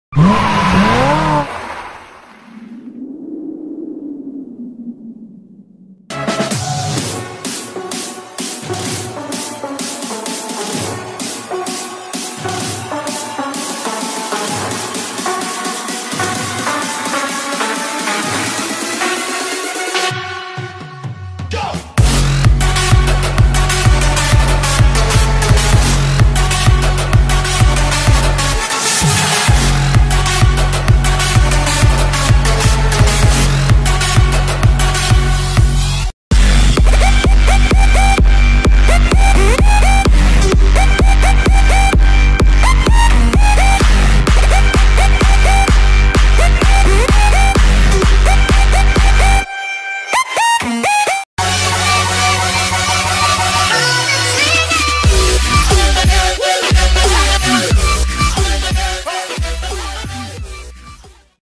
HONDA CITY 2002 | TIGER | LOADED | ELECTRO MOTORSPORTS | PAKWHEELS KARACHI AUTO SHOW 2018
Loaded Honda City 2002 from Electro Motorsports at PakWheels Karachi 2018.